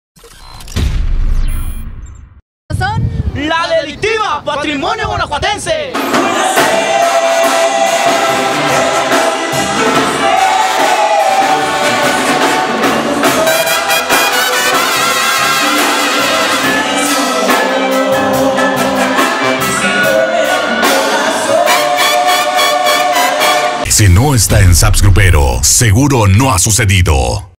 Banda La Delictiva Patrimonio Guanajuatense (Convención ADEEM, Puebla 2018)
Banda La Delictiva "Patrimonio Guanajuatense", estuvo presente en la convención ADEEM, Puebla 2018; acá un resumen.